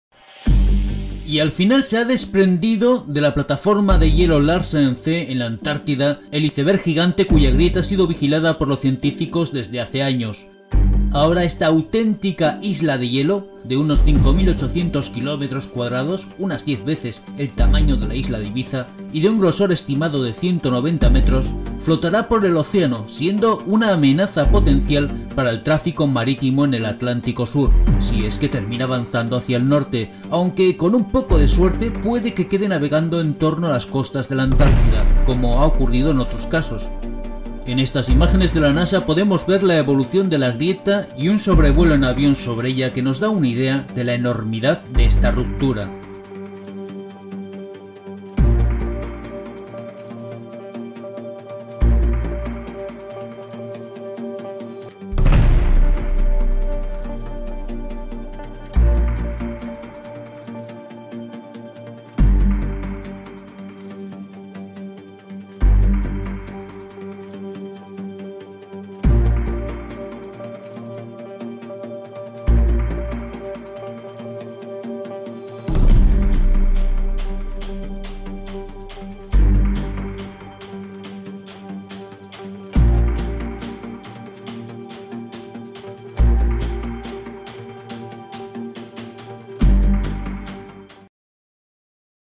Sobrevuelo del iceberg gigante del "billón de toneladas" desprendido de la Antártida
ExoPlanetas.com Noticia: Y al final se ha desprendido de la Antártida el iceberg gigante cuya grieta ha sido vigilada por los científicos desde hace años. Ahora esta auténtica isla de hielo de unos 5800 kilómetros cuadrados, unas 10 veces el tamaño de la isla de Ibiza, y de un grosor estimado de unos 190 metros, flotará por el océano, siendo una amenaza potencial para el tráfico marítimo en el Atlántico Sur, si es que termina avanzando hacia el norte; aunque con un poco de suerte puede que quede navegando en torno a las costas de la Antártida, como ha ocurrido en otros casos.
El iceberg desprendido estos días de la Antártida de 5800 km2 no es el mayor registrado en la historia. El récord lo tiene el B-15, que se desprendió en el año 2000 y tenía 11.000 km2. Todavía hay 8 fragmentos (uno de ellos de 270 km2) de esta isla de hielo flotando cerca del continente helado.
En estas imágenes de la NASA podemos ver la evolución de la grieta y un sobre vuelo en avión sobre ella que nos da una idea de la enormidad de esta ruptura.